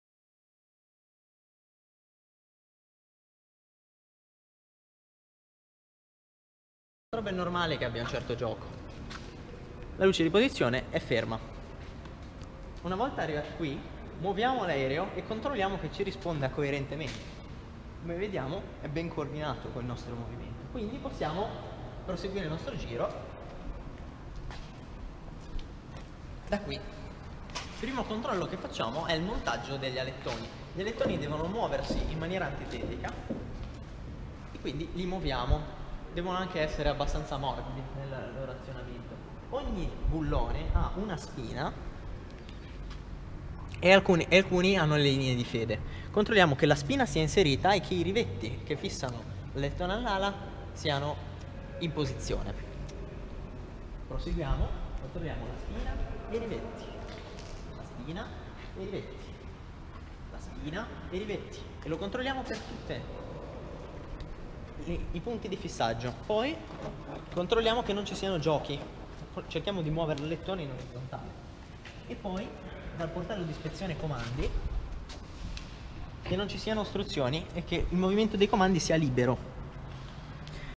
5 Step Controlli pre volo
Un allievo effettua i controlli pre volo.